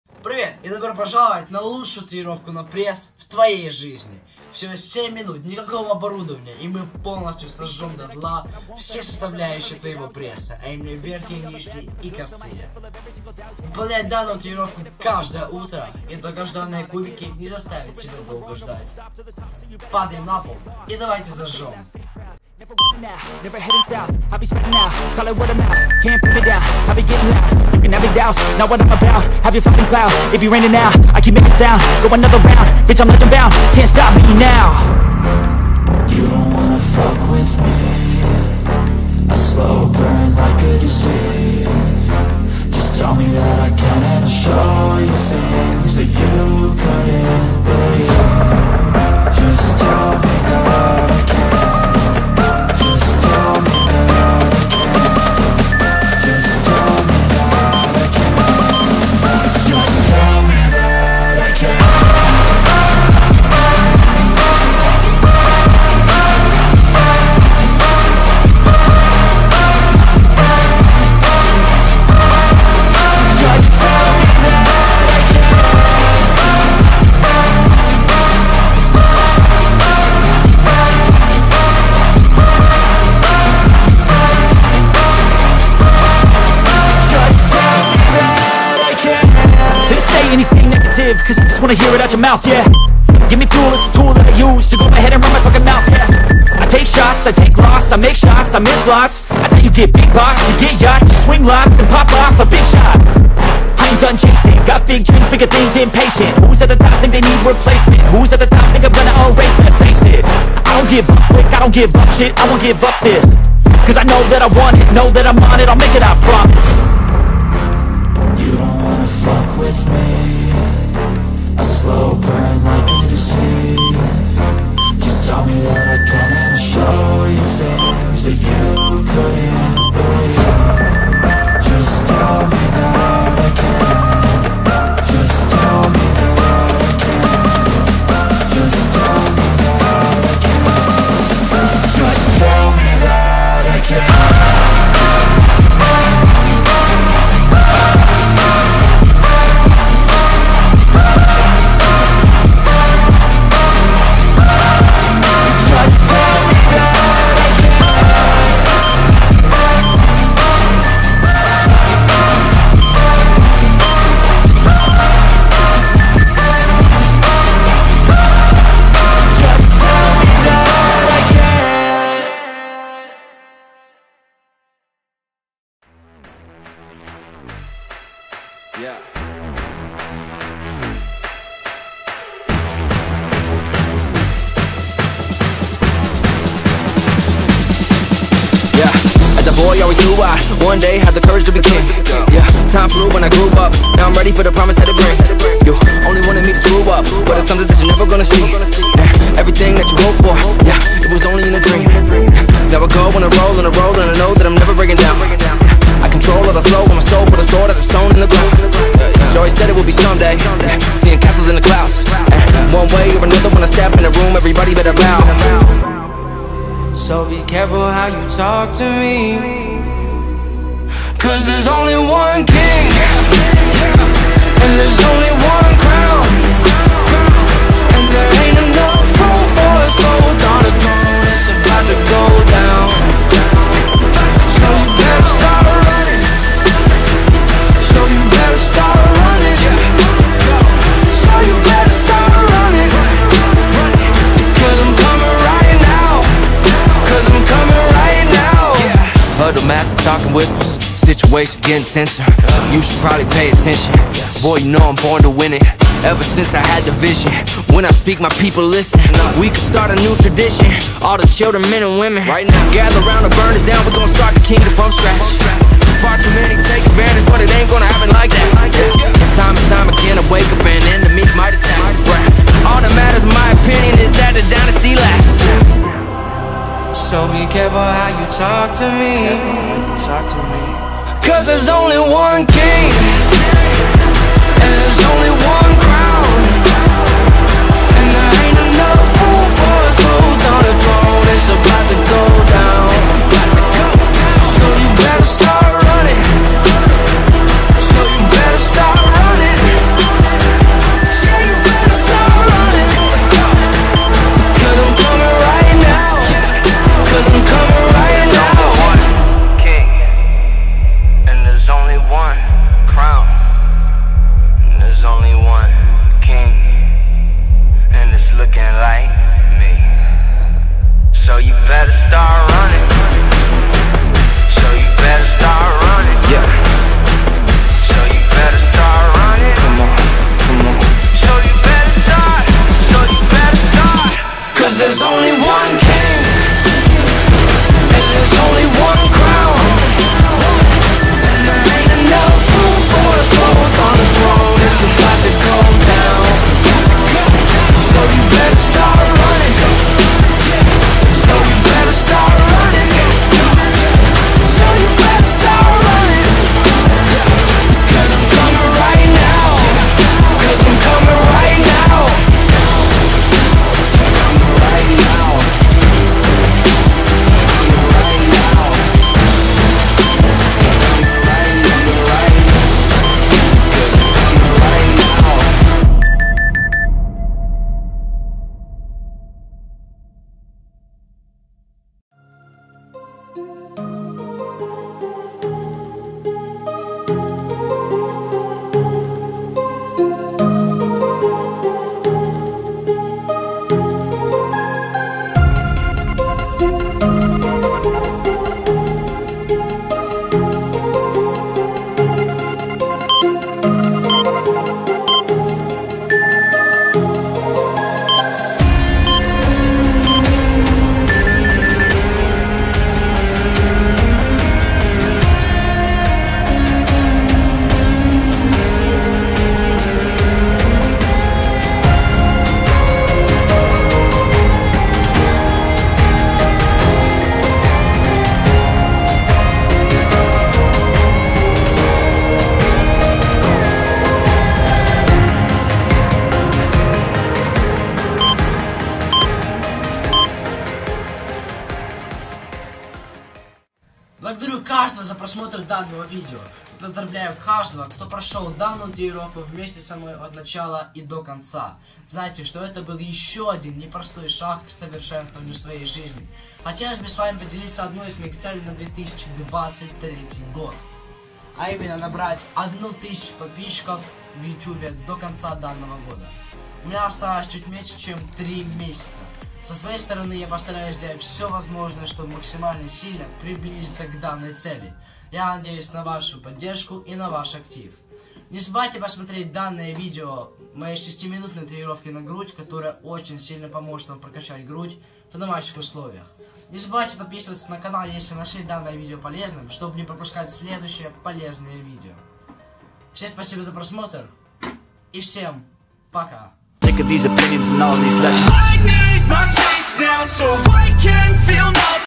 Получи Пресс За 7 МИНУТ В ДЕНЬ! (100% ГАРАНТИРОВАННО!) В 2023
Выполняй данную тренеровку каждое утро, и твой пресс станет стальным! (не забудь о питании!)

122/1000